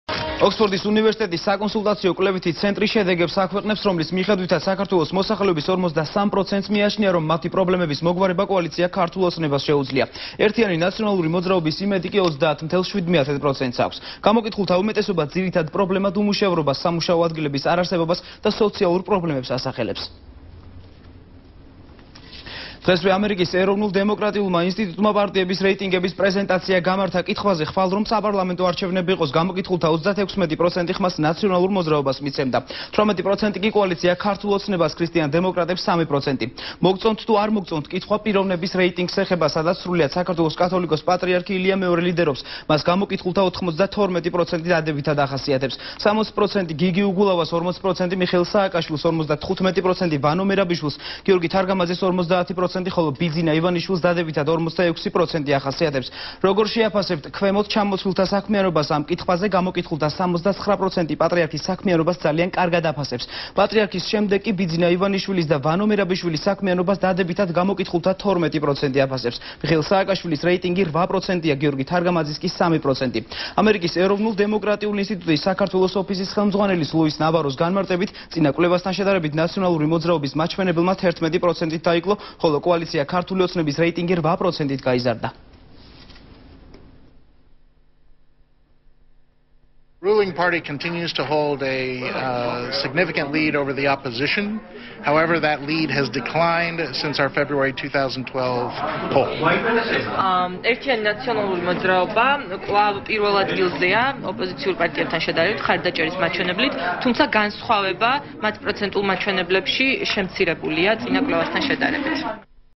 ახალი 3 | ოქსფორდის კვლევა | 16.07.12
ოქსფორდის  უნივერსიტეტის საკონსულტაციო კვლევითი ცენტრი შედეგებს აქვეყნებს, რომლის მიხედვითაც  საქართველოს მოსახლეობის 43%-ს მიიჩნია, რომ მათი პრობლემების მოგვარება კოალიცია "ქართულ ოცნებას" შეუძლია, "ერთიანი ნაციონალური მოძრაობის" იმედი - 30.7 %-ს აქვს. გამოკითხულთა უმეტესობა ძირითად პრობლემებად უმუშევრობას, სამუშაო ადგილების არარსებობას და სოციალურ პრობლემებს ასახელებს.